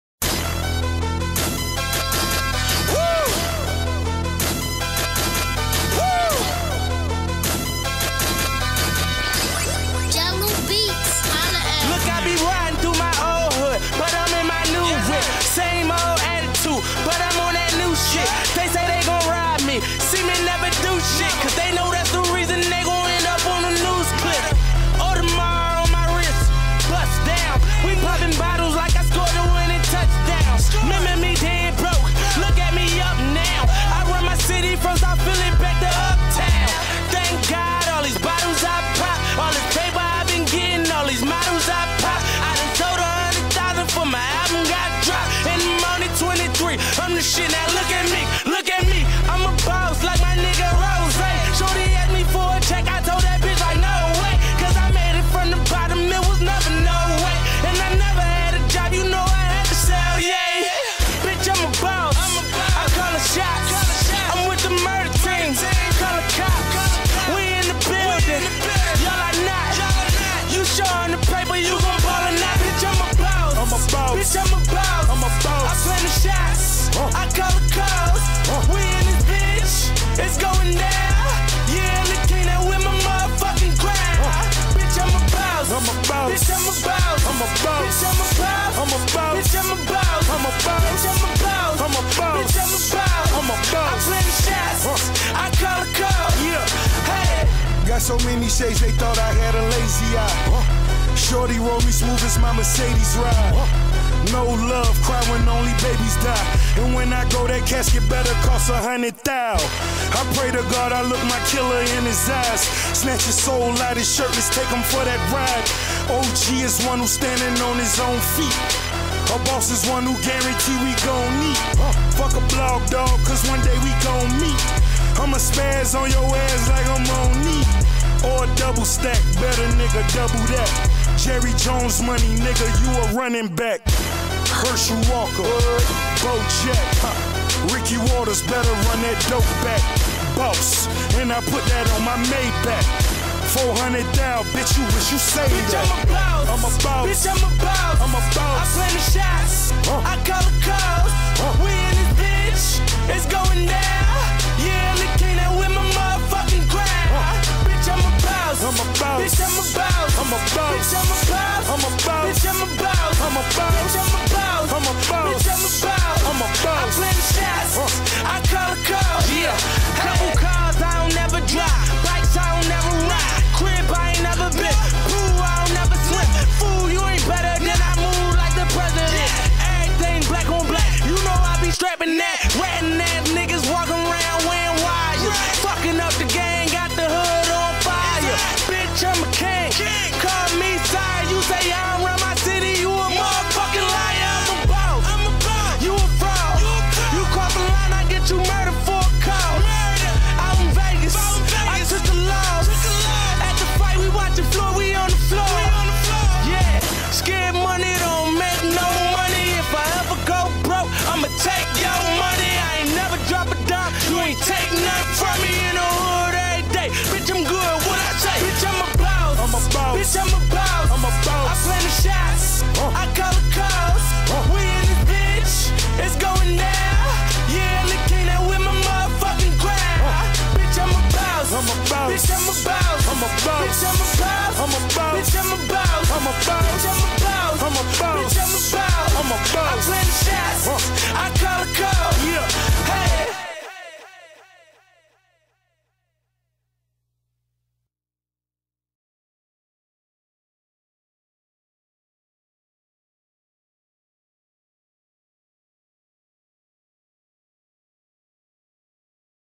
kyle slater 12 touchdowns 3 games
Against winton woods(Ended up 13-2 State Champs!), Lakota West (9-1), and Middetown(10-1). Anderson beat ww 52-38, lw 24-21, and mid 41-20.